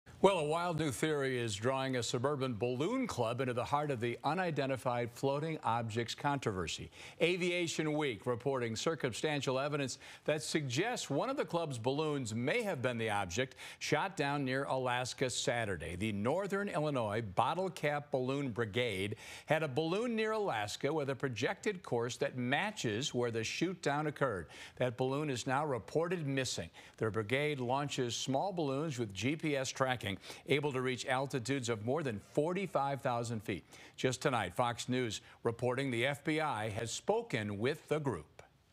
Balloon club may have had balloon shot down by USAF
A wild new theory is drawing a balloon club into the heart of the unidentified floating objects controversy. STORY